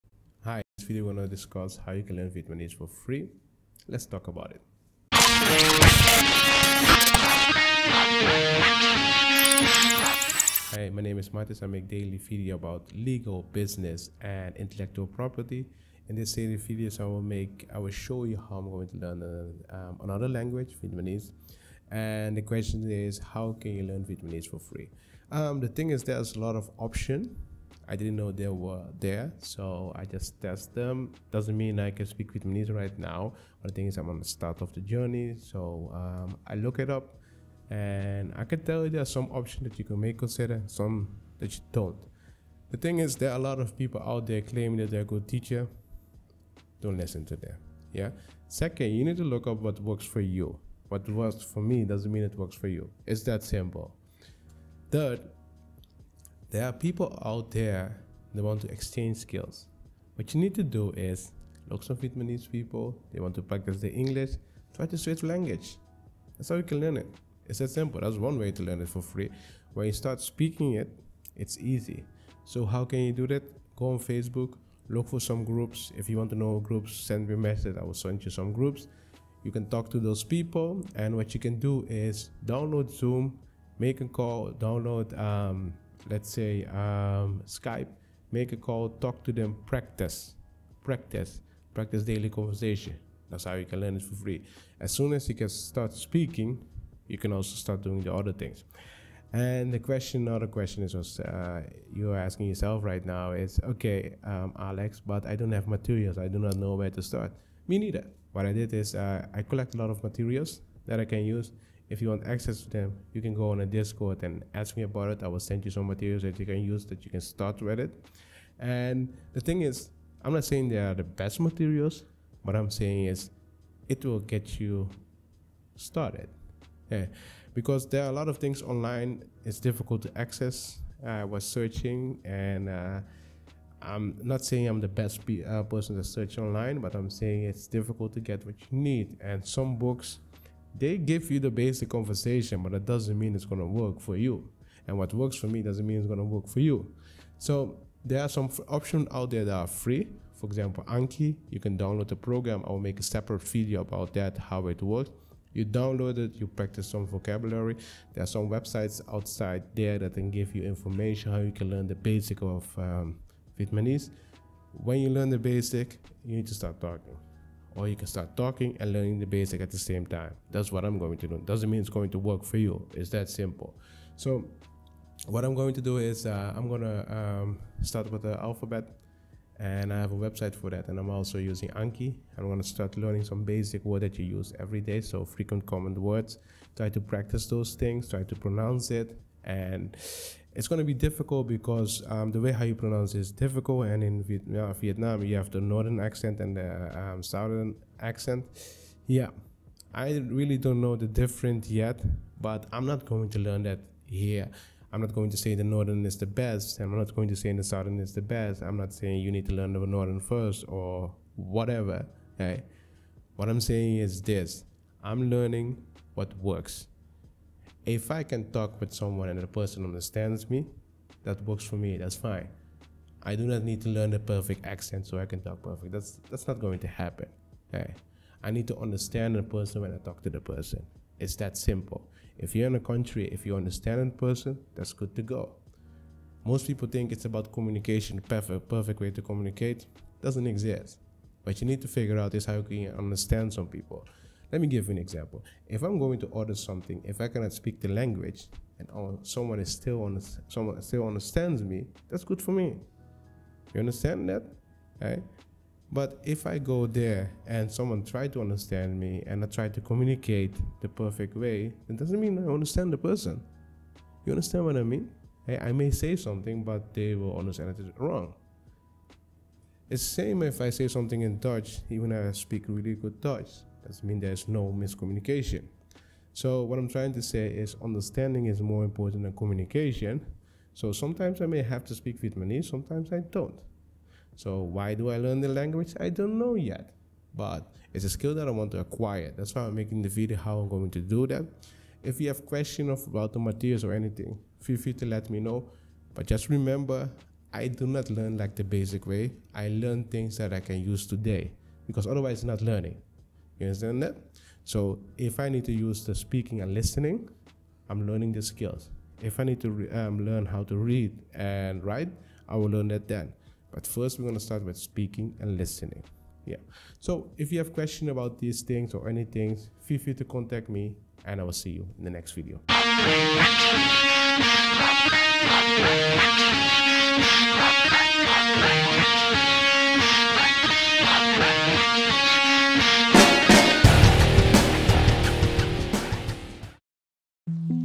How to learn the Vietnamese language for free? | Martisz Legal
In today's video, we're discussing the following question: How to learn the Vietnamese language for free? This video will be very helpful for beginners.

Official Websites